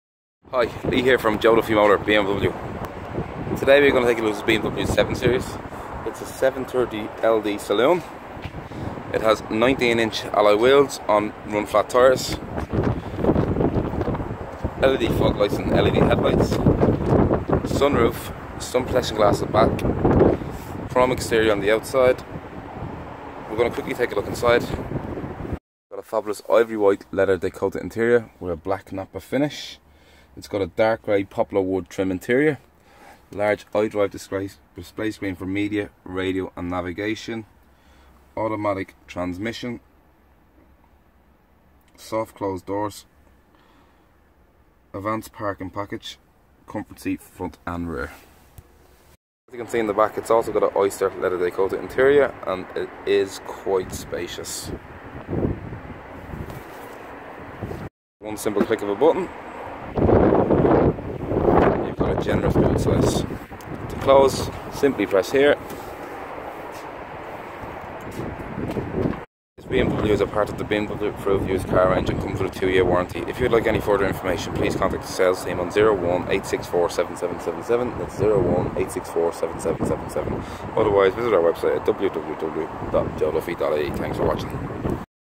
181D5107 - 181D5107 BMW 730Ld Saloon
Here is a video of our 181D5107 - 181D5107 BMW 730Ld Saloon. Please contact Tom Hayes to arrange a test drive today. Joe Duffy BMW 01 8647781